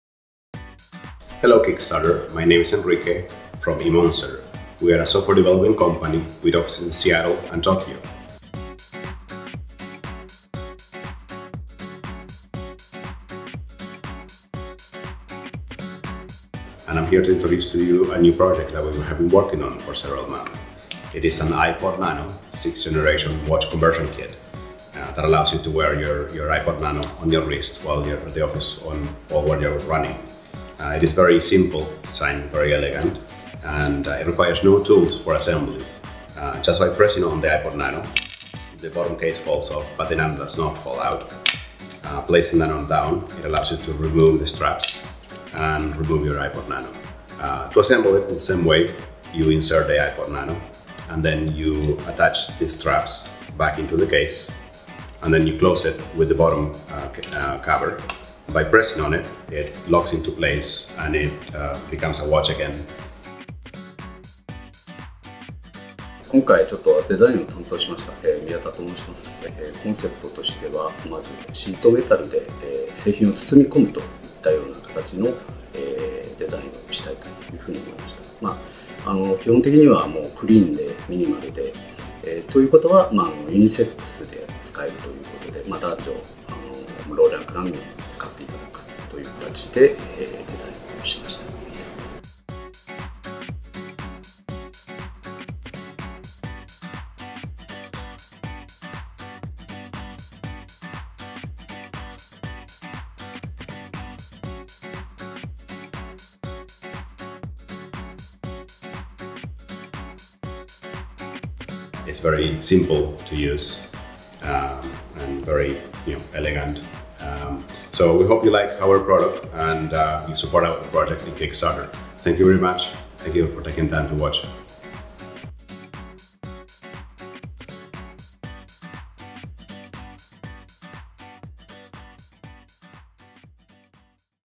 nanox from your friendly emonster
nanox - Apple iPod nano 6th Gen Watch Conversion Kit by emonster (nanox.emonster.com)